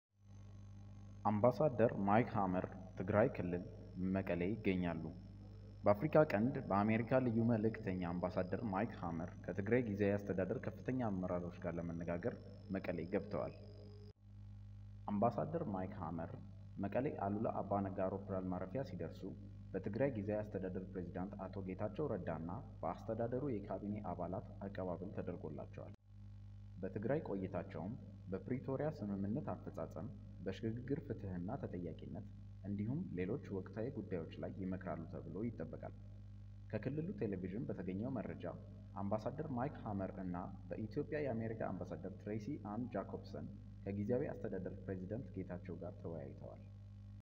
Ethiopia - የአሜሪካው ልዩ መልእክተኛ በመቀሌ | ትግራይ ምን እየተደረገ ነው !!! | ትኩስ ዜና
አምባሳደር ማይክ ሀመር ትግራይ ክልል፣ መቐለ ይገኛሉ።

በአፍሪካ ቀንድ የአሜሪካ ልዩ መልዕክተኛ አምባሳደር ማይክ ሀመር ከትግራይ ጊዜያዊ አስተዳደር ከፍተኛ አመራሮች ጋር ለመነጋገር ዛሬ መቐለ ገብተዋል።

አምባሳደር ማይክ ሀመር መቐለ አሉላ አባነጋ አውሮፕላን ማረፊያ ሲደረሱ በትግራይ ጊዜዊ አስተዳደር ፕሬዝዳንት አቶ ጌታቸው ረዳና በአስተዳደሩ የካቢኔ አባላት አቀባበል ተደርጎላቸዋል።

በትግራይ ቆይታቸውም በፕሪቶሪያ ስምምነት አፈፃፀም፣በሽግግር ፍትሕና ተጠያቂነት እንዲሁም ሌሎች ወቅታዊ ጉዳዮች ላይ ይመክራሉ ተብሎ ይጠበቃል።

ከክልሉ ቴሌቪዥን በተገኘው መረጃ ፤  አምባሳደር ማይክ ሀመር እና በኢትዮጵያ የአሜሪካ አምባሳደር ትሬሲ አን ጃኮብሰን ከጊዜያዊ አስተዳደር ፕሬዝዳንት ጌታቸው ረዳ ጋር  ተወያይተዋል።